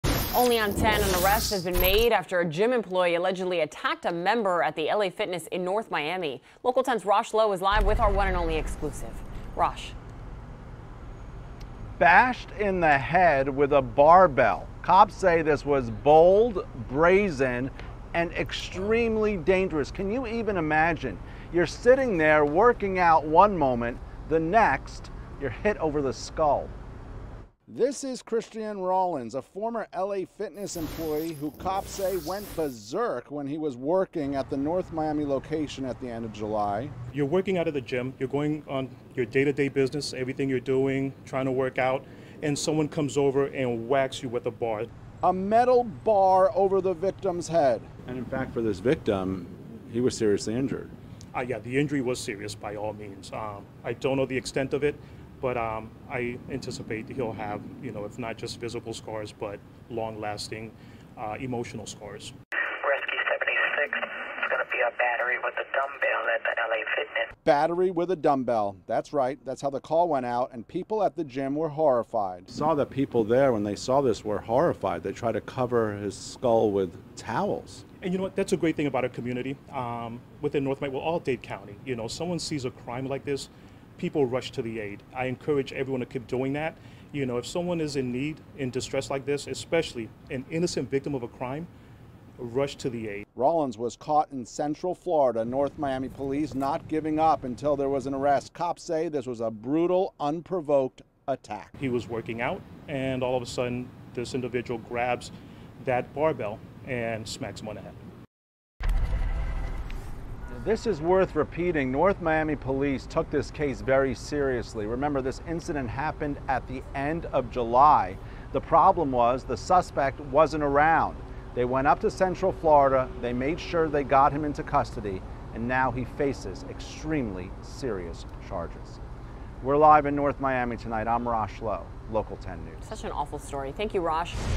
Gym employee arrested for attacking member with barbell
Police officers arrested a man for using a barbell to hit a person in the head. The man was an employee of a gym and the victim was a member of the gym.